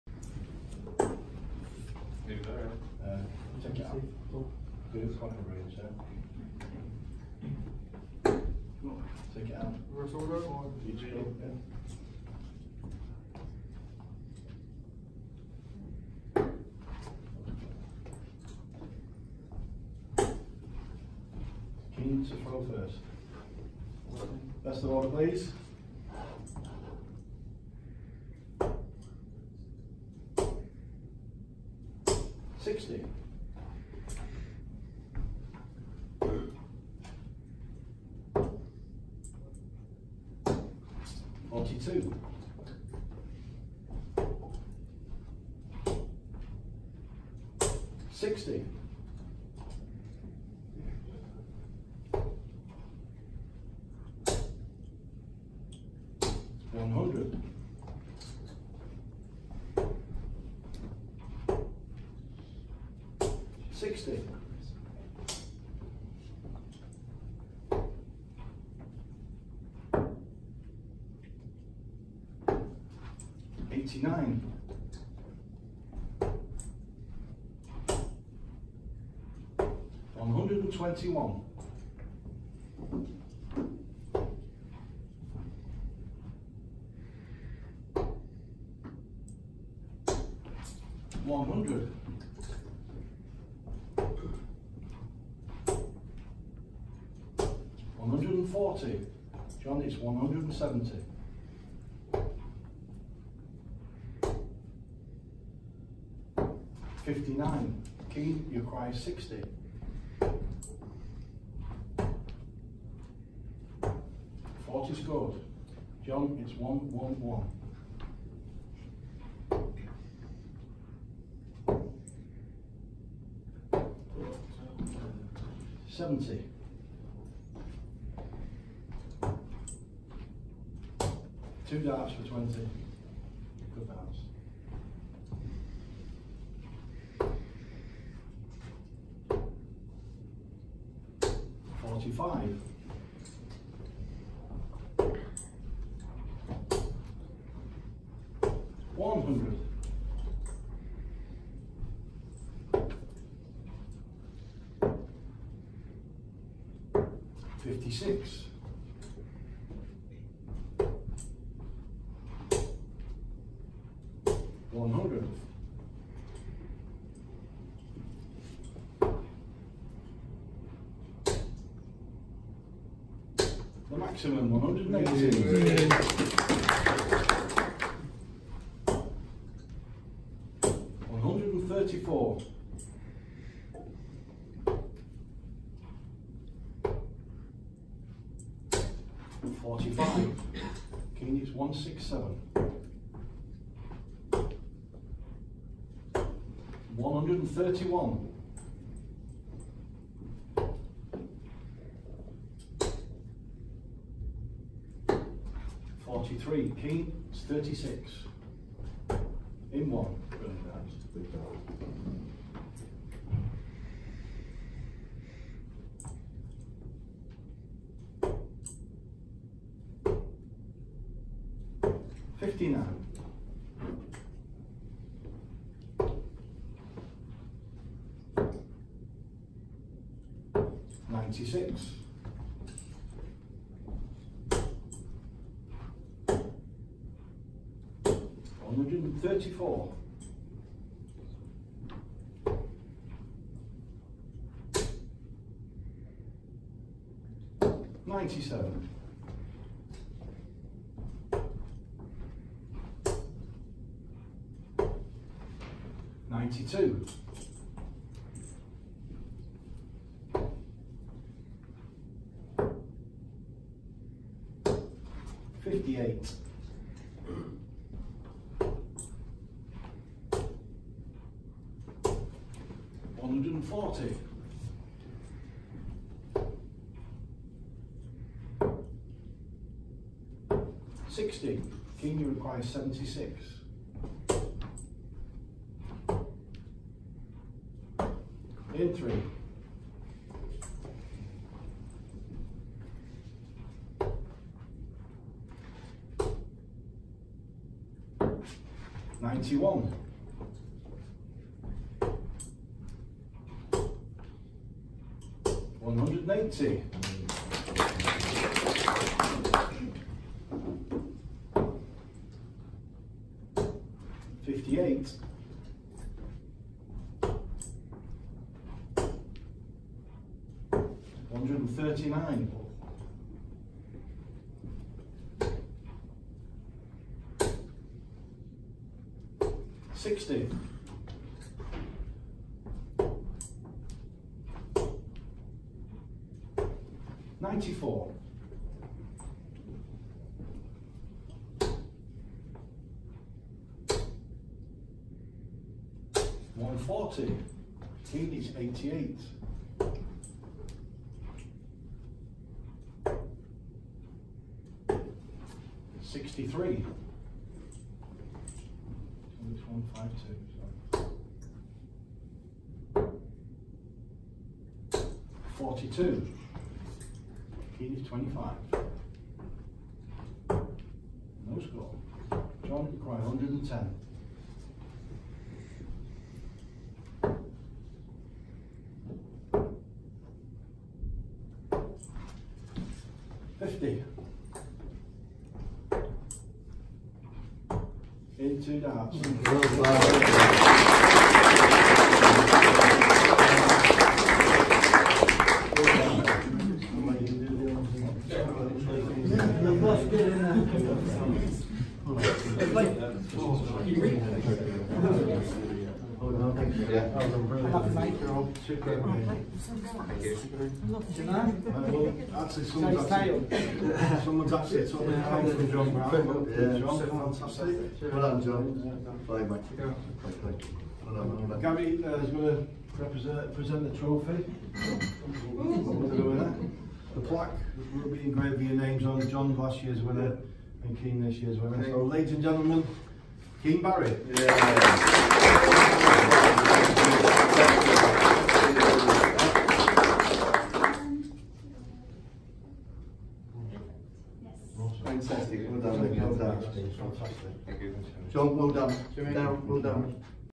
Elite 1 Training Day 2019
We pulled together the Elite 1 programme so that we could discover the next generation of pro darts players, and work with the best of the best youth players to help them, alongside their management, to develop their talents to tour card level. Lead by Darryl Fitton, Rob Cross and Lorraine Winstanely joined us for a day spent of training the group and ended with a 501 competition between Keane Barry and John Brown.